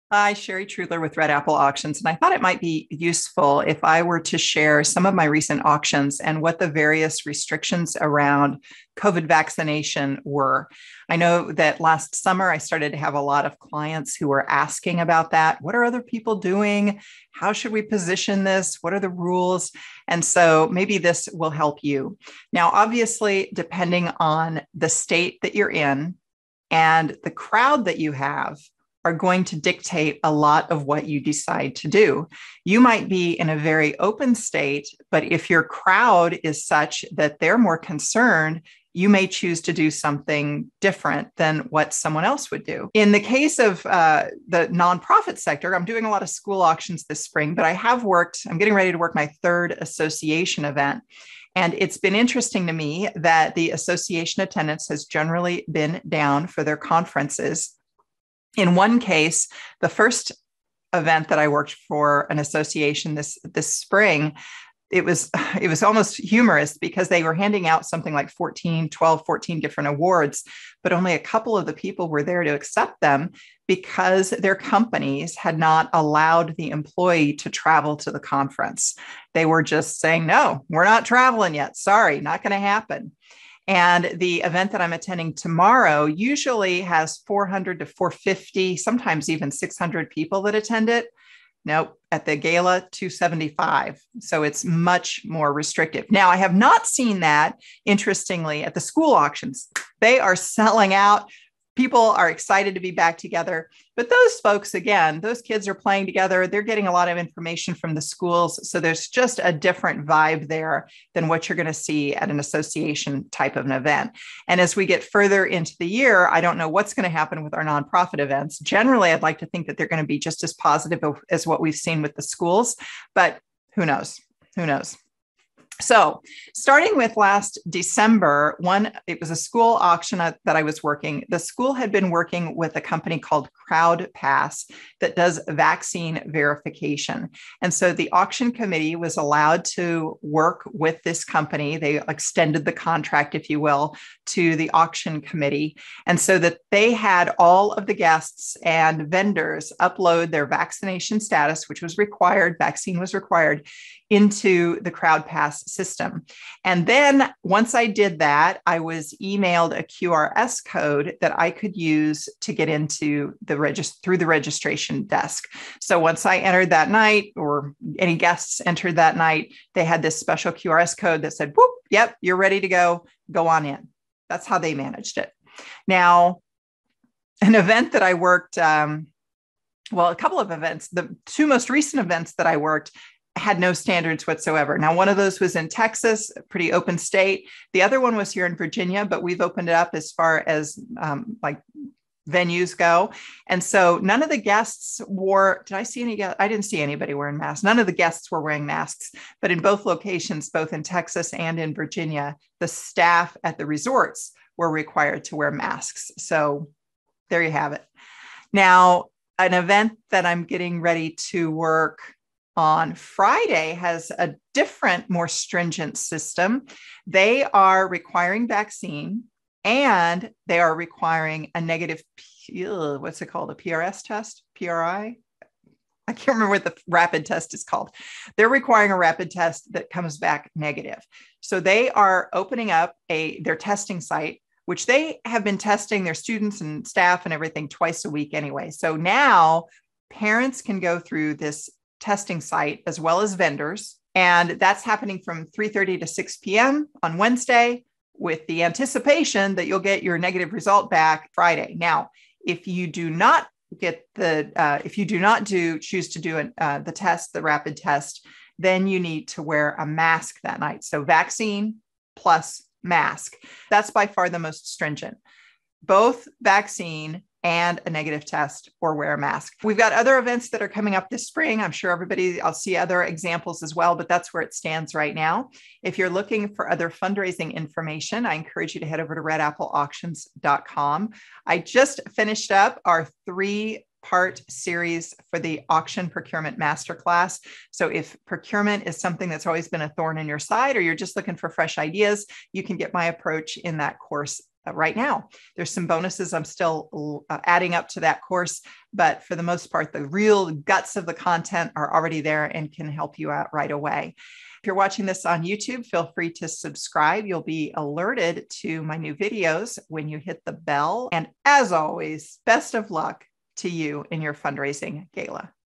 Handling COVID safety precautions at your fundraising auction
How are you handling COVID precautions at your gala?  Whether you are in an 'open' state or one that is more cautious, supporters want to know what they should expect at your event.

Consider devoting a page to your gala website to COVID requirements.

* If you won't be doing anything -- tell them.
* If you expect them to upload a vaccination card, proof of booster, negative test, and wear a N-95 respiration mask -- tell them.
* If you're going to have comfort-level bracelets available -- tell them.
* If no one is required to be masked but it's OK if they wear one (and you'll have masks available) -- tell them.

My point: Be clear.

People can make their own decisions as to whether they want to attend or not, but they need to understand what your event will "look like" before they buy a ticket.

In this video, I share what four recent fundraising auctions did for COVID requirements.